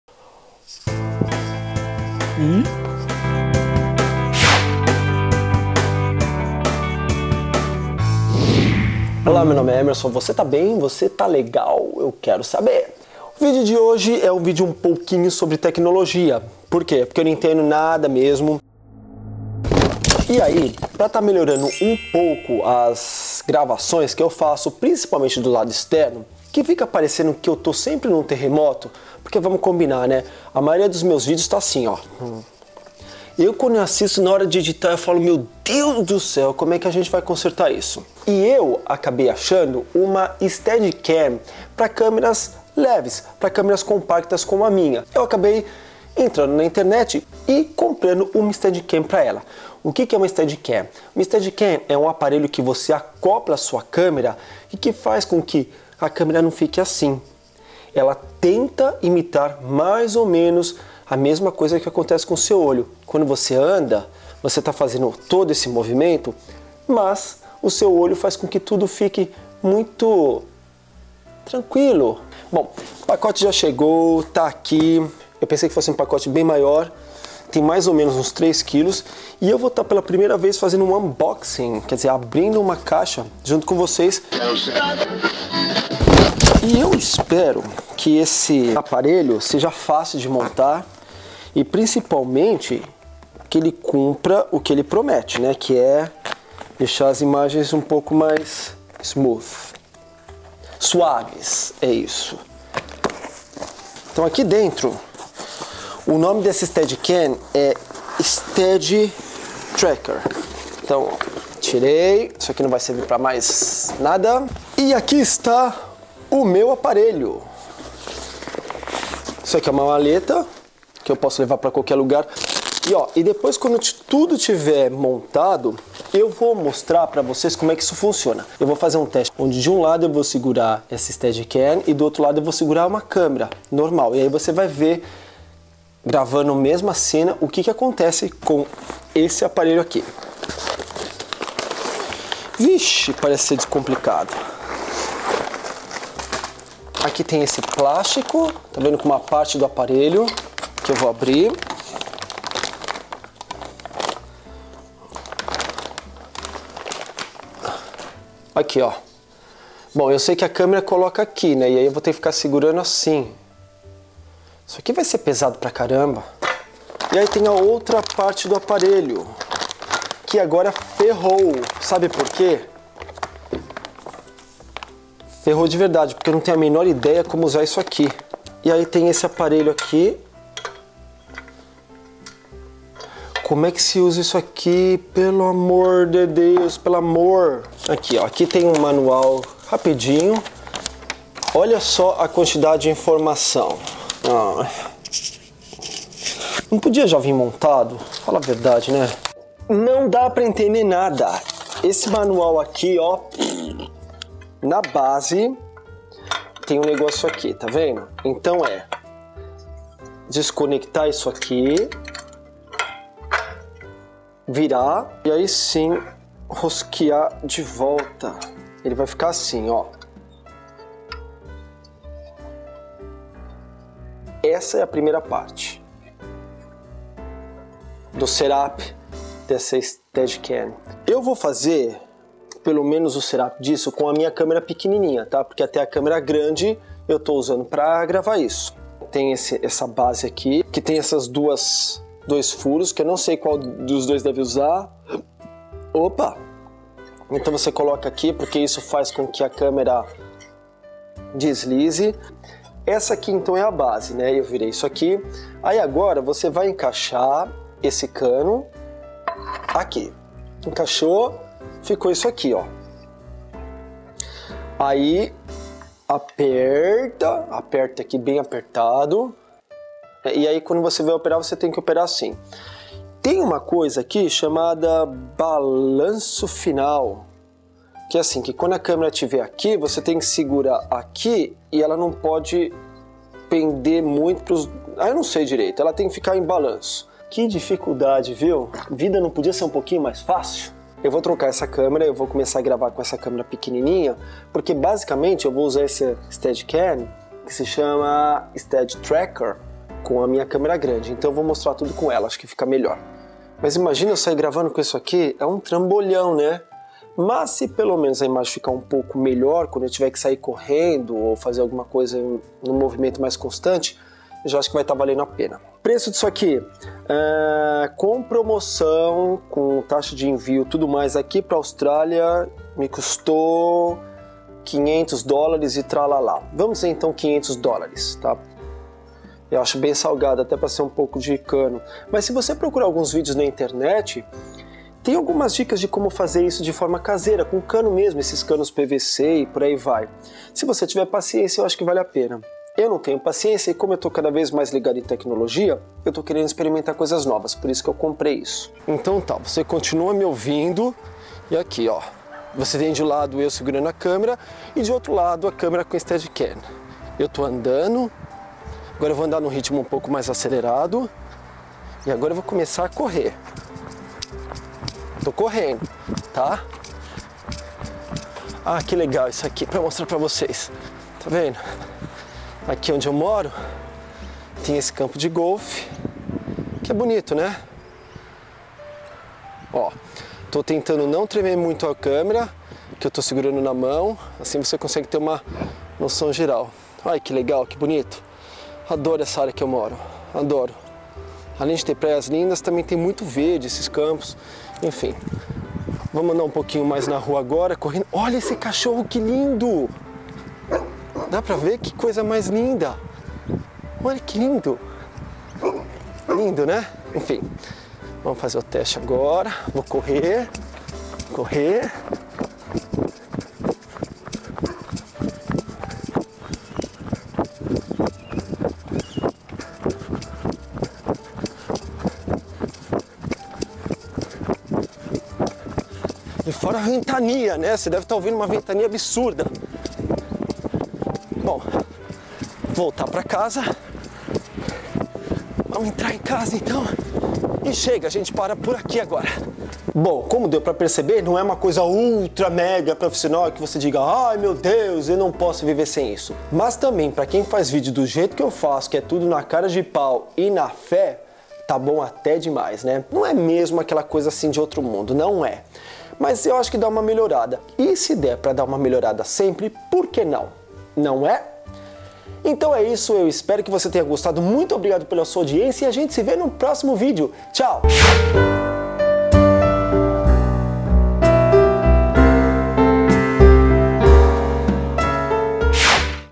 Abrindo um trambolho (Steadicam) - EMVB - Emerson Martins Video Blog 2012
Pela primeira vez o Tio Emerson resolveu abrir uma encomenda que chegou pelo correio e dividir esse momento lindo com todos vocês.

A geringonça que ele comprou foi uma Steadicam. Trata-se de um aparelho que estabiliza a imagem de uma câmera em extremo movimento.
Como tudo pra ele é muito difícil, levou horas tentando montar um aparelho super básico e aproveitou pra mostrar o que o aparelho promete!

Divirta-se e fique bem informado!

Namastê Bitch!